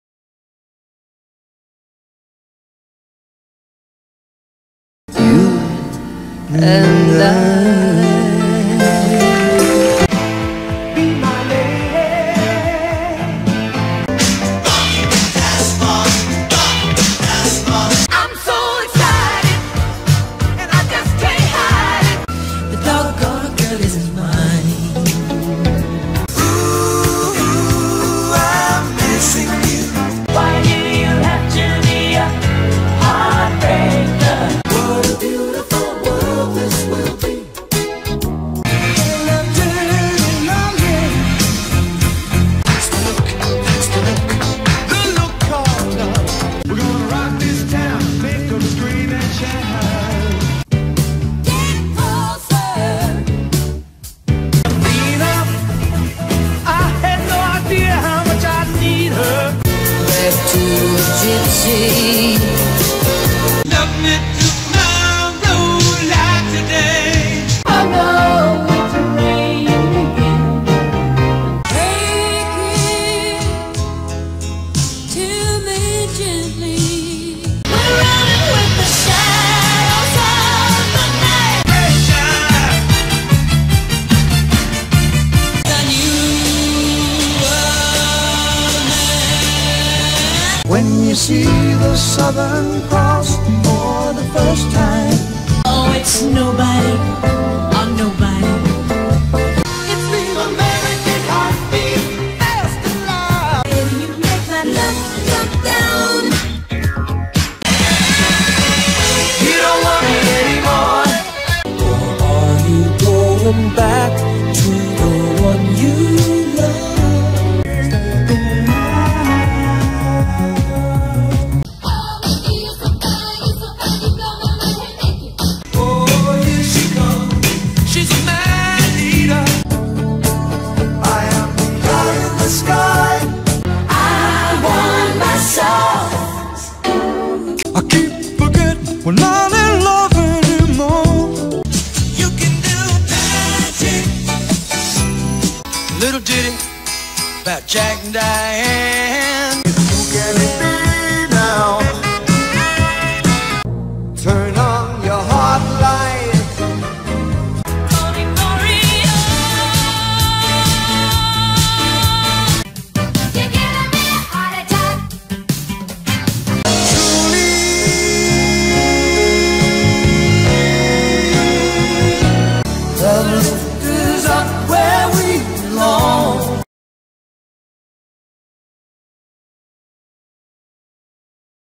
Billboard Top 40 Hits - November 13, 1982
The Top 40 Songs on the Billboard Hot 100 Chart for the week ending November 13, 1982.